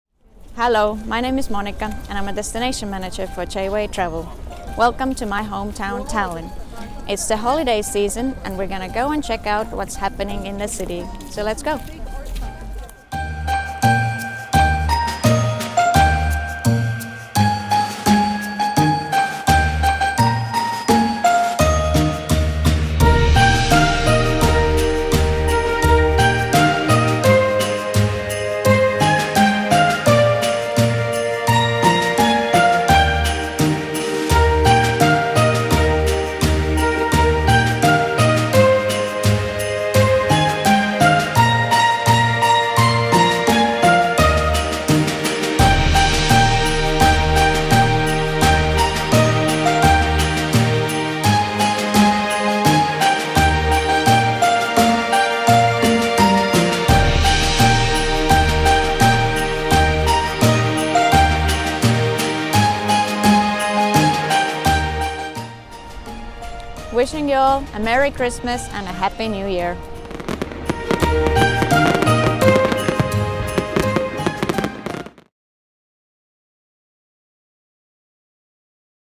Tallinn's Christmas Markets
Tallinn was voted best Christmas Market in Europe this year after previous three-time winner Zagreb had to sit out a year. Tallinn's got some Christmas history behind it too, as it claims to have invented the custom of placing an official Christmas tree in the main square, back in 1441.

Moonika, our resident Estonian, created this video to give you a taste of what Tallinn is like at Christmas time these days.

Being so far north, snow is a distinct possibility, so if you like the full seasonal experience, head to the Baltics